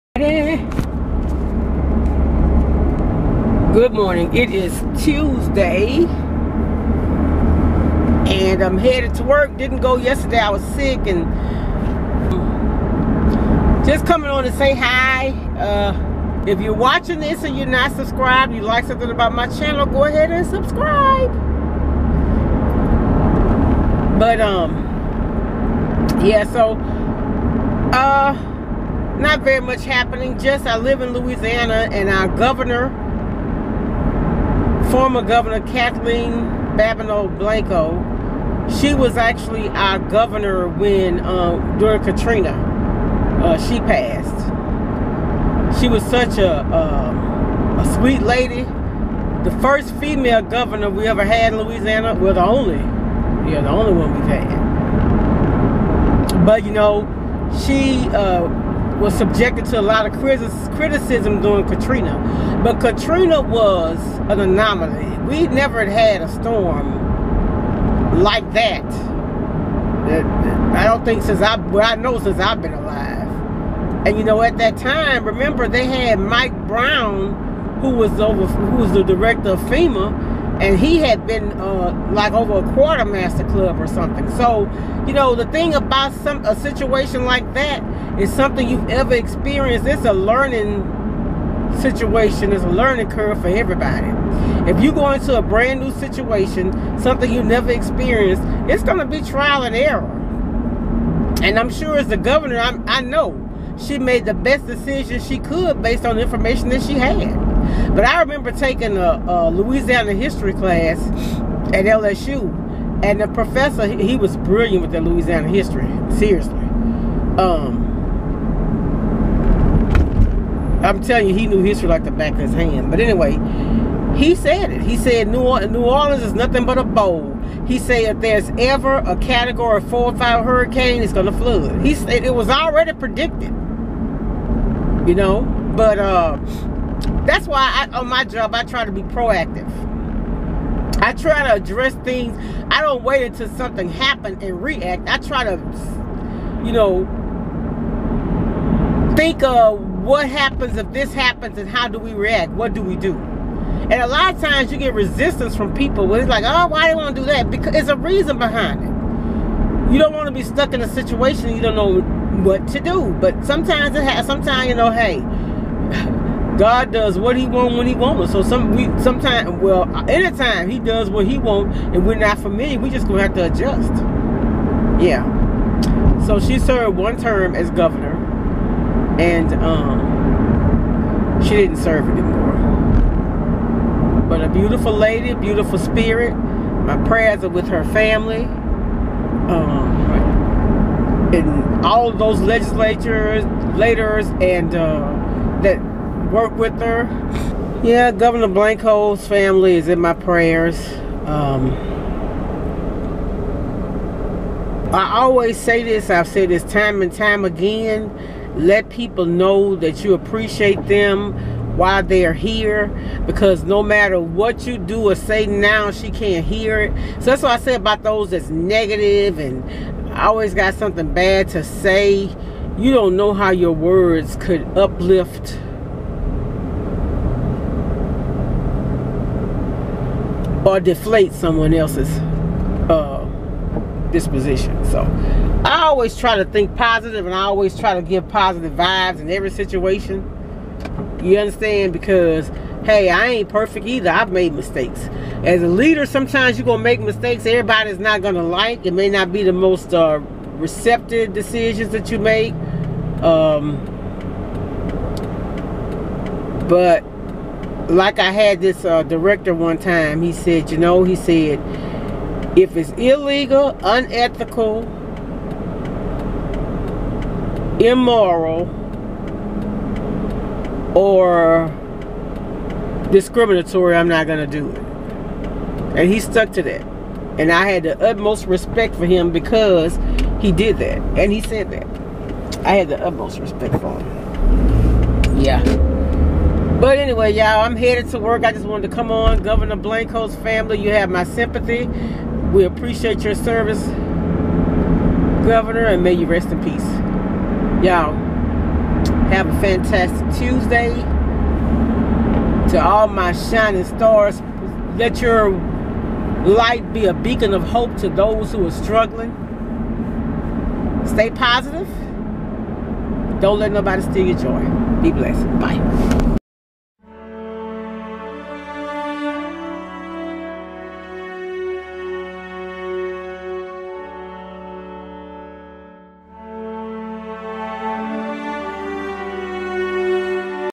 R I P FORMER GOVERNOR BLANCO
I would like to offer prayers of comfort and send condolences to the family and the State of Louisiana in the death of our Former Governor Kathleen Babineaux Blanco.

Have you lifted ANYONE this week? You STILL have time. ANY act of kindness can uplift someone.

ALWAYS remember that! A mere smile can brighten someone's day! I've TRIED it and IT"S uplifted ME!